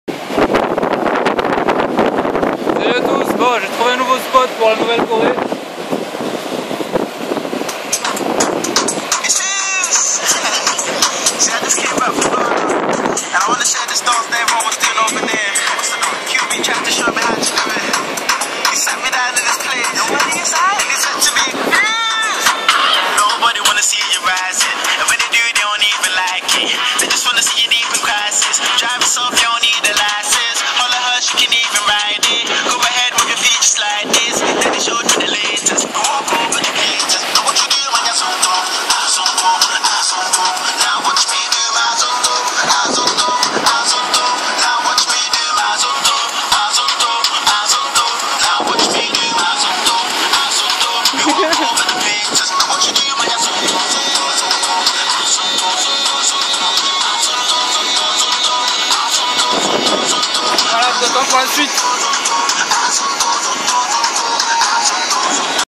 Zumba azonto
Teaser de la chorée zumba "azonto",
 soleil, plage,  musique,  vacances et zumba
En attendant la prochaine vidéo.